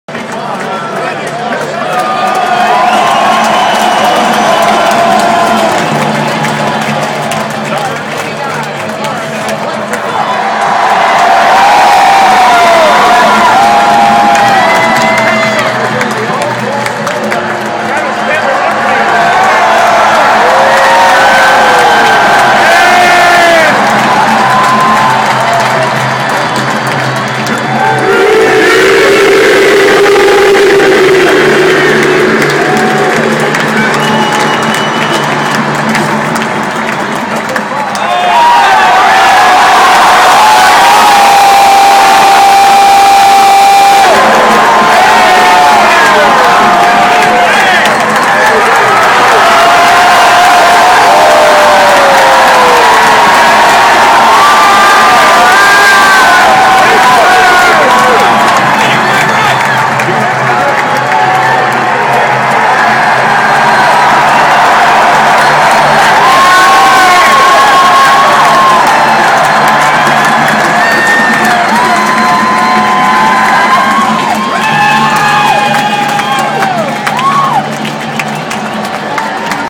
Baltimore Ravens offense announcement
Baltimore Ravens offense pregame announcement from section 112 against the Steelers 11/29/09.